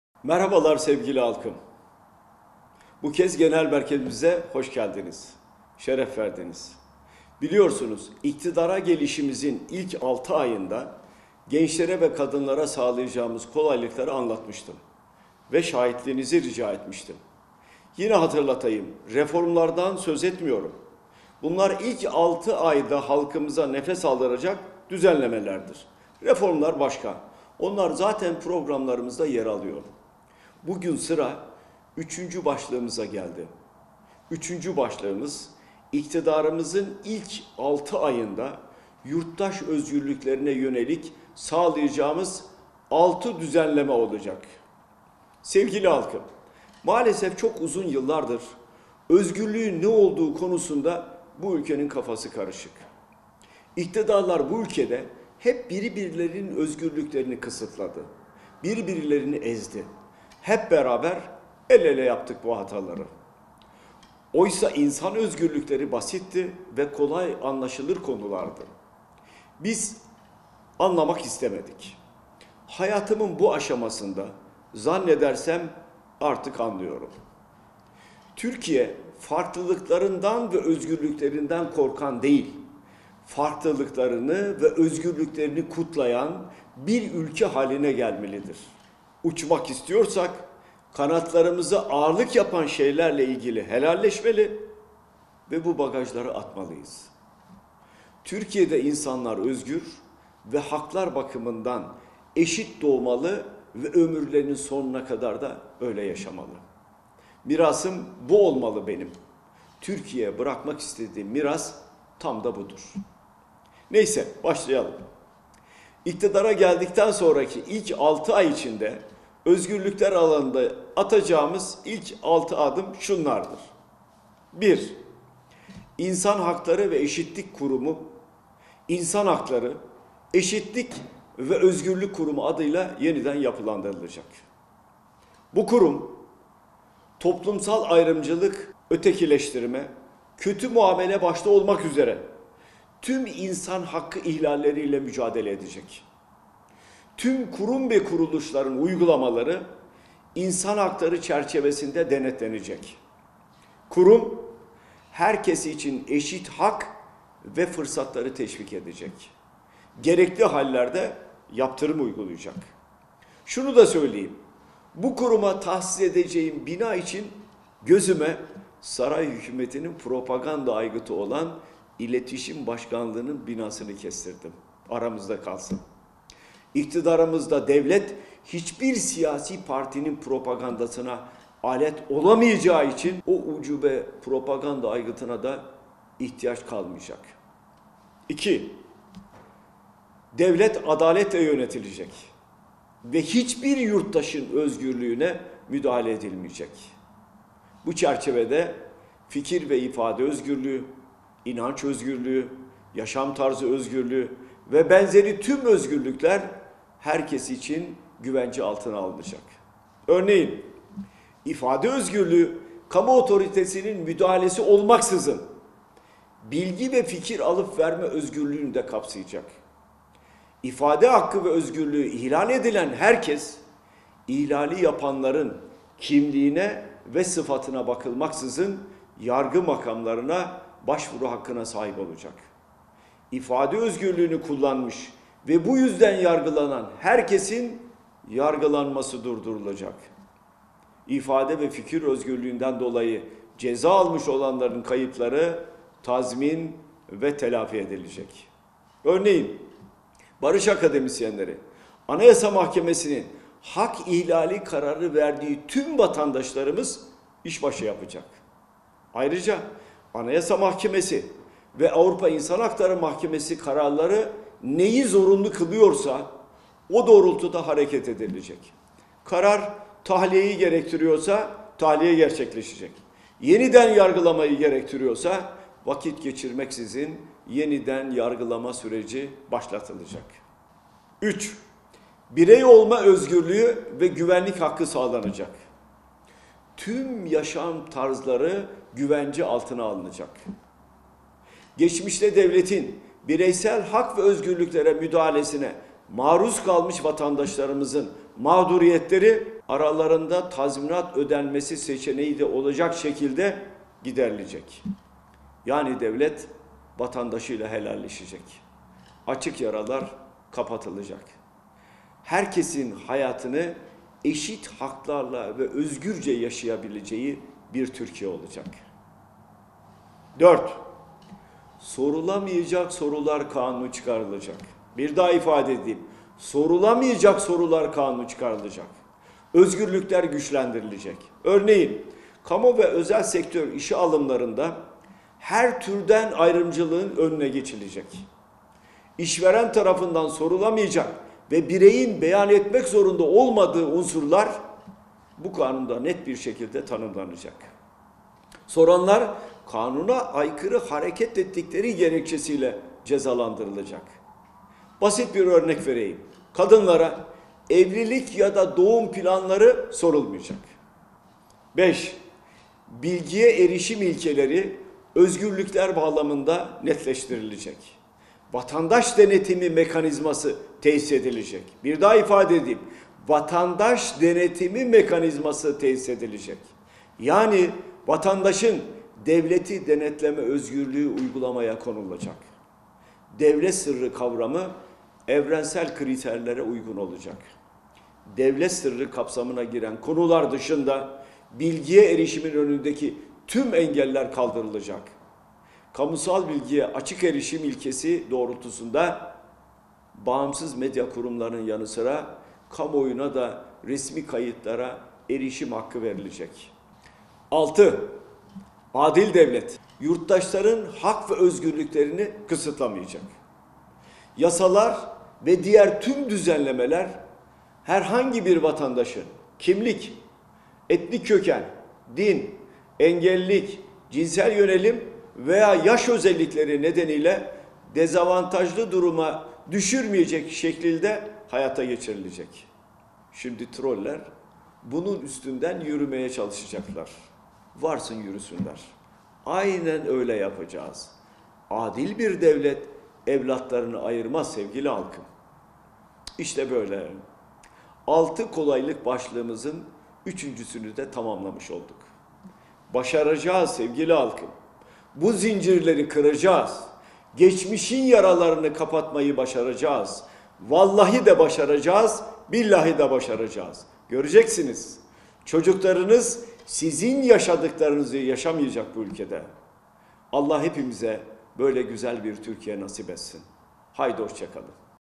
Kemal Kılıçdaroğlu: Atacağımız özgürlük adımlarını açıklıyorum.
CHP Genel Başkanı Kemal Kılıçdaroğlu, iktidara geldiklerinde ilk 6 ayda atacakları özgürlük adımlarına ilişkin açıklama yaptı. Kılıçdaroğlu, özgürlük adımlarına ilişkin, şunları ifade etti:‘‘ Türkiye farklılıklarından ve özgürlüklerinden korkan değil, “farklılıklarını ve özgürlüklerini kutlayan” bir ülke haline gelecek. Bu videomda iktidarımızın ilk 6 ayında atacağımız özgürlük adımlarını açıklıyorum. Biraz uzun ama önemli:) Milletimizi şahit olmaya davet ediyorum...‘‘